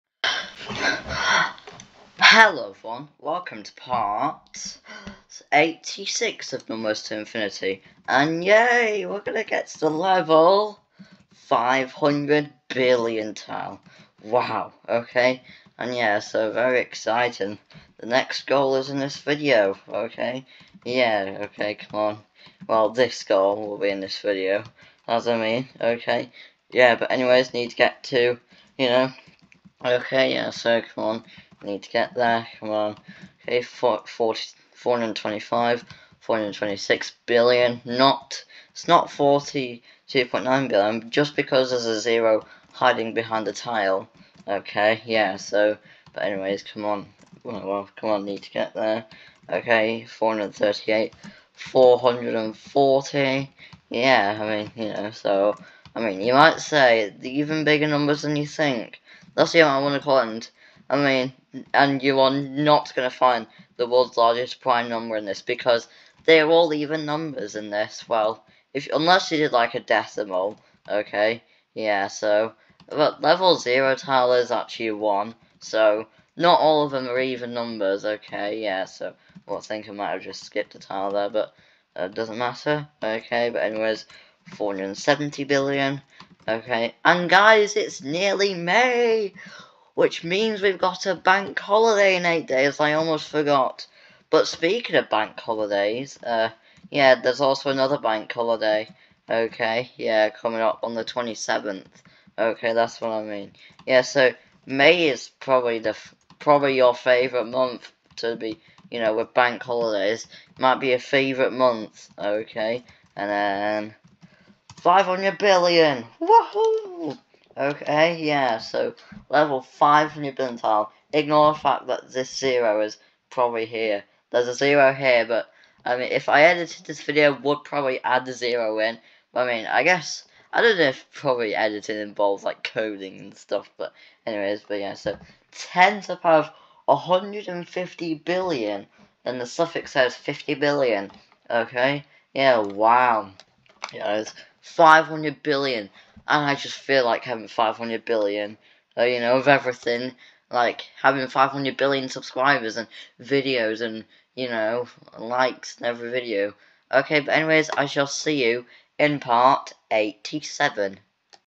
Numbers To Infinity Part 86
It's Part 86 of Numbers To Infinity and yes, level 500B tile